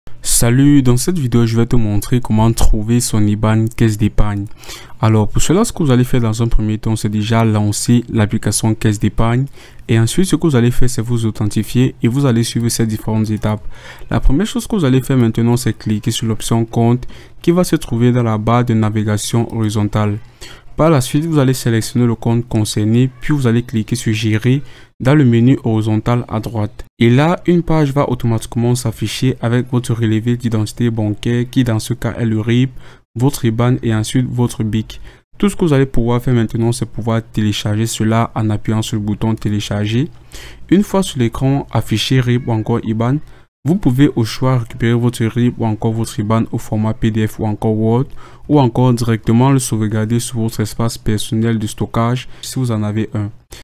Comment Trouver Son IBAN Caisse D'Épargne ( Nouvelle Méthode 2025 )
Dans cette vidéo , je vais te montrer Comment Trouver Son IBAN Caisse D'Épargne .

Je te montre dans cette vidéo Comment Trouver Son IBAN Caisse D'Épargne.

A la fin de cette vidéo , tu sauras Comment Trouver Son IBAN Caisse D'Épargne.

À BIENTÔT 😉